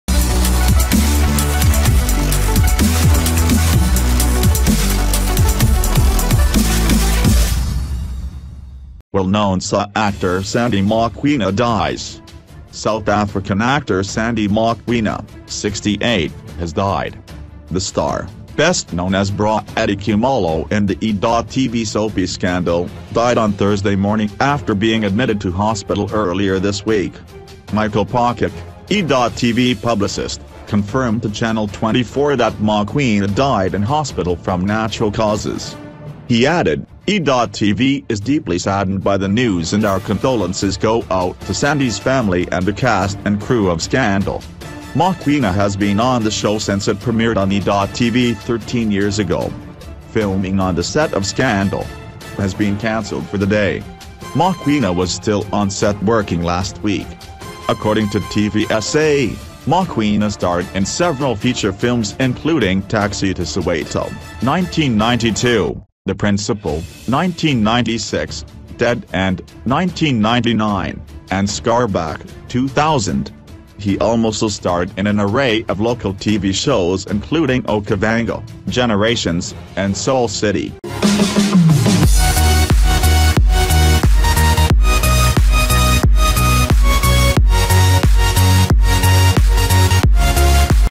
Well-known SA actor Sandy Mokwena dies
Well-known SA actor Sandy Mokwena dies. South African actor Sandy Mokwena, 68, has died. The star, best known as Bra Eddie Khumalo in the e.tv soapie Scandal!, died on Thursday morning after being admitted to hospital earlier this week.